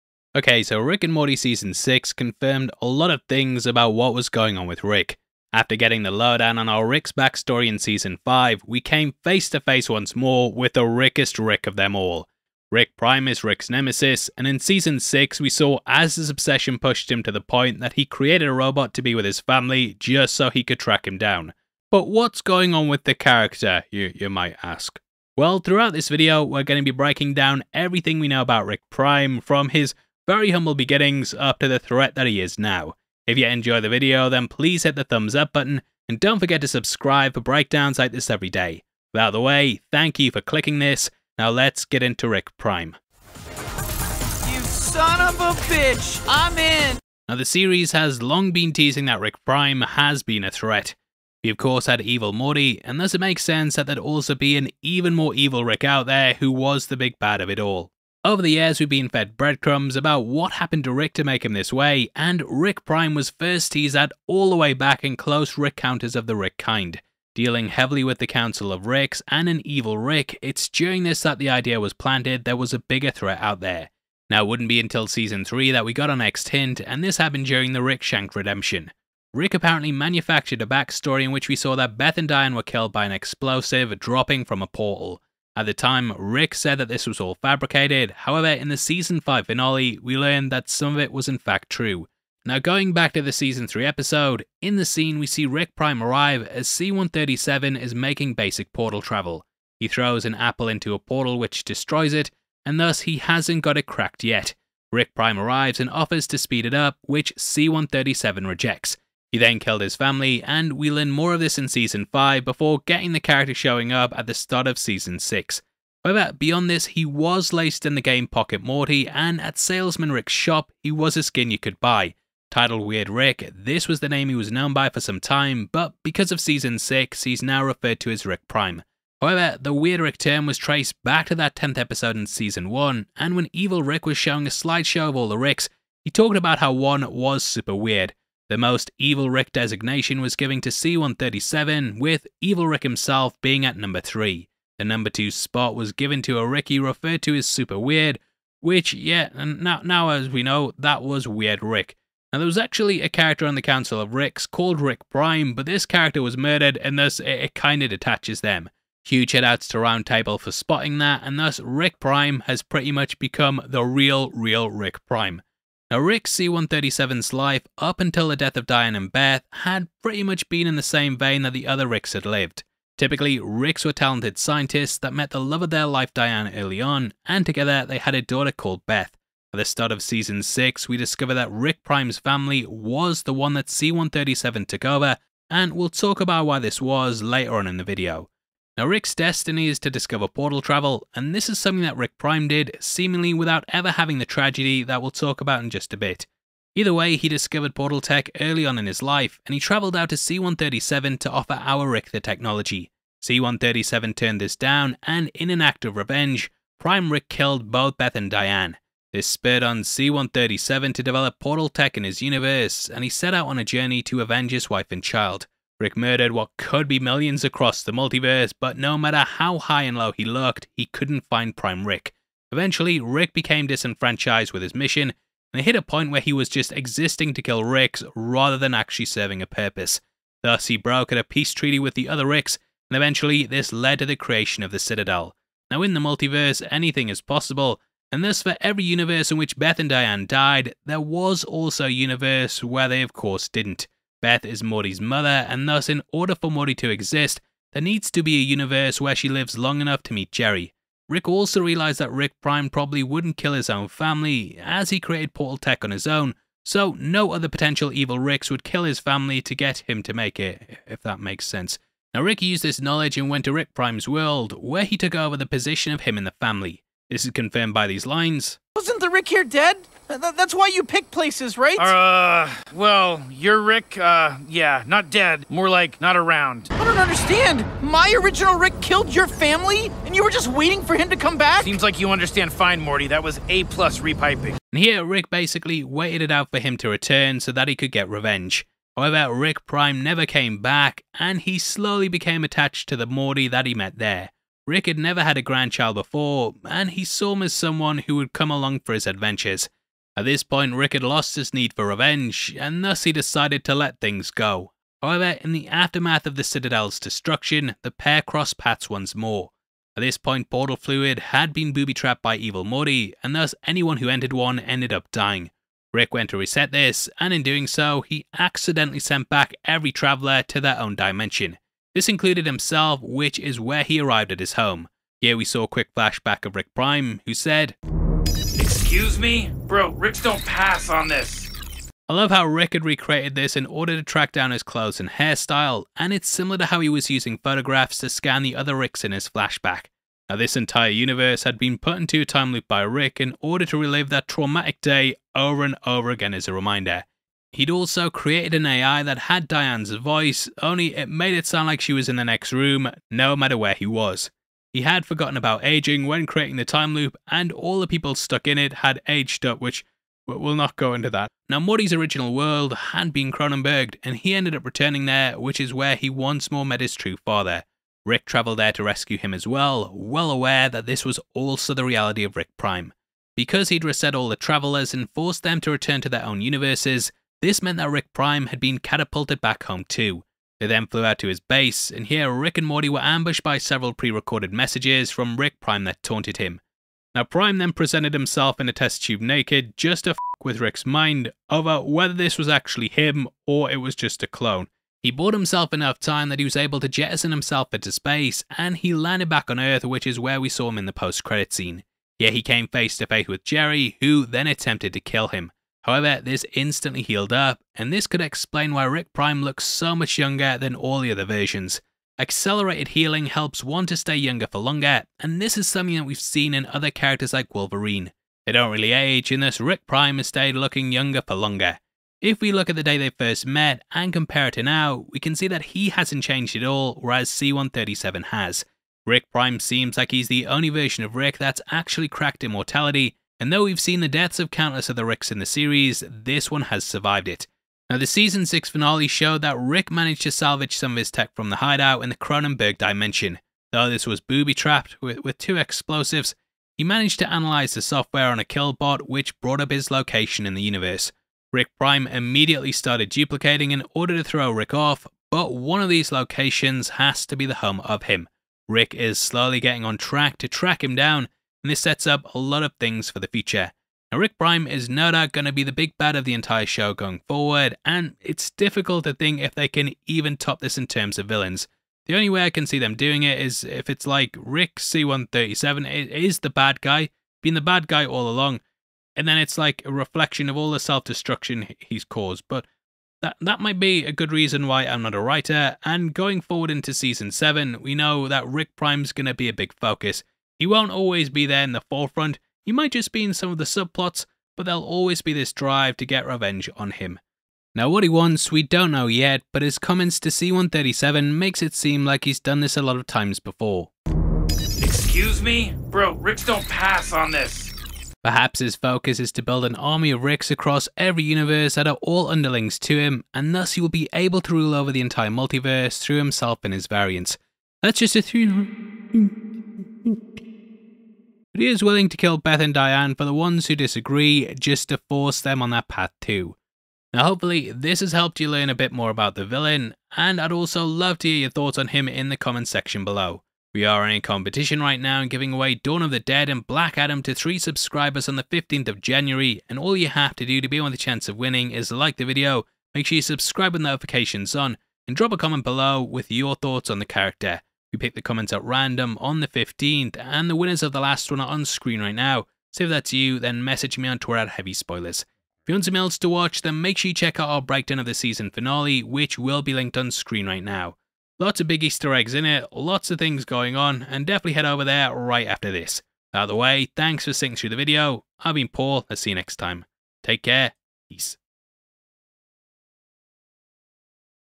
RICK AND MORTY Rick Prime Explained | Character Breakdown and What We Know About The TRUE Villain
RICK AND MORTY Rick Prime Explained | Character Breakdown And Everything We Know About The TRUE Villain.  After getting confirmation of our Rick's backstory in Season 5 we came face to face once more with the Rickest Rick of them all.  Rick Prime is Rick's nemesis and in Season 6 we saw his obsession push him to the point that he created a Robot to be with his family just so he could track him down. Throughout this video we're gonna be breaking down everything we know about RICK PRIME and also giving our theories on what we think will happen with him, lots to get through so let's get into Rick Prime.

Get some awesome Heavy Spoilers show clothes, phonecases and accessories

/* SOCIAL MEDIA */

/* VIDEO INFORMATION */

Ok so Rick and Morty Season 6 confirmed a lot of things about what was going on with Rick. After getting confirmation of our Rick's backstory in Season 5 we came face to face once more with the Rickest Rick of them all. That is Rick Prime. A goddamn sumoffabich that murdered Beth and Diane and sent Rick off on the quest of revenge in which he countless versions of himself across the multiverse. Rick Prime is Rick's nemesis and in Season 6 we saw as his obsession pushed him to the point that he created a Robot to be with his family just so he could track him down.

But what's going on with the character?

Well throughout this video we're gonna be breaking down everything we know about the character and also giving our theories on what we think will happen with him. With that out the way, thank you for clicking this, now let's get into Rick Prime.

Now the Series has long been teasing that Rick Prime has been a threat. We of course had Evil Morty and thus it makes sense that there'd also be an even more evil Rick out there. Over the years we've been fed breadcrumbs about what happened to Ricks to make him this way and Rick prime was first mentioned all the way back in Close Rick-Counters of the Rick Kind. Dealing heavily with the council of Ricks and an Evil Rick it's during this that the idea was planted that there was a bigger threat out there.